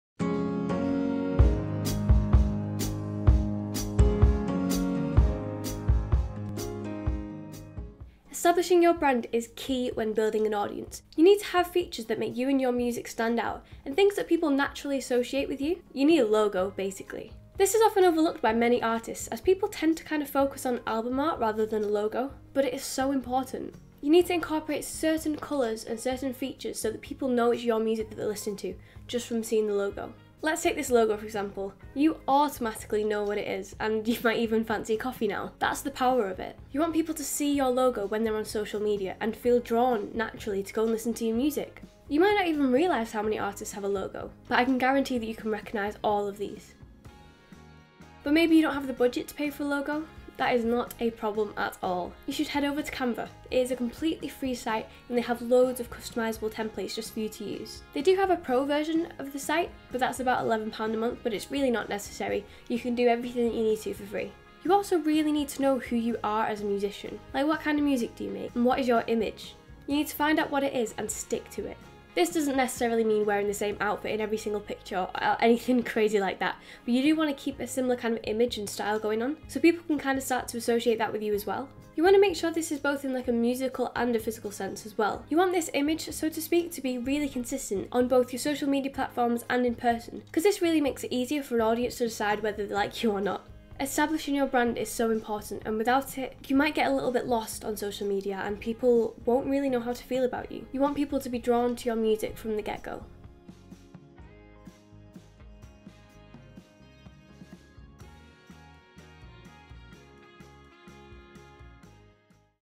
FREE SOCIAL MEDIA COURSE | Episode 1: Establishing Your Brand
I've recently released a free course on how to use Social Media and wanted to share it here with you!

It is very loosely catered towards musicians but the knowledge can be applied to any niche!

If you would like any of the downloadable content mentioned, please visit the course here:

Let me know if you have any video requests in the comments below!